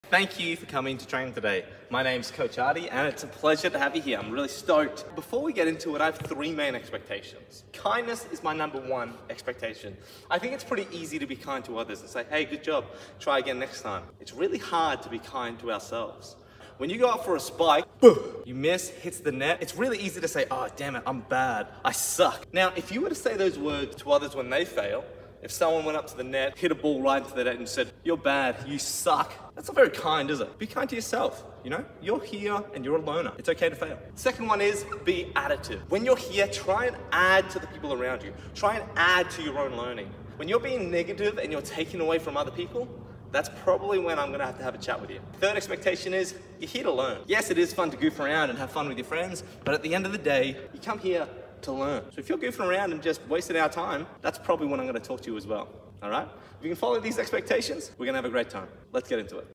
POV - Your first training session with coach Artie
These are my expectations for my players, my coaches, the parents, and myself. I hope our volleyball community can buy into these expectations as well.

Edited out my dramatic pauses to fit into a minute.